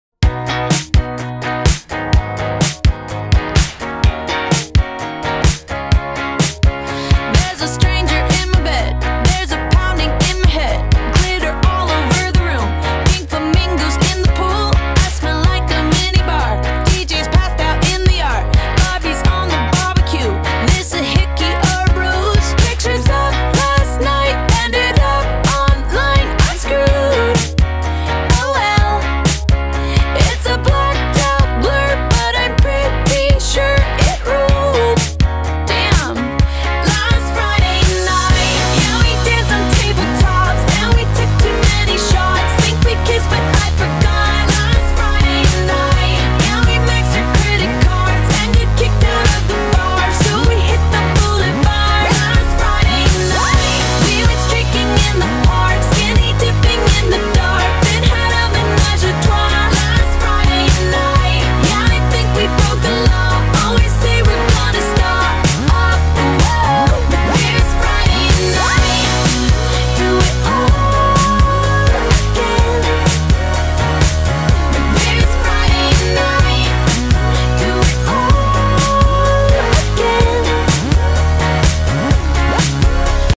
Friday Pillow Party Barbie - 2017 IDC Extra doll
unveiling video of the official "Friday Pillow Party" Barbie, exclusive extra doll for the 2017 Italian Doll Convention in Milan, designed by Magia2000 and dresses manufactured by Tatiana Storai.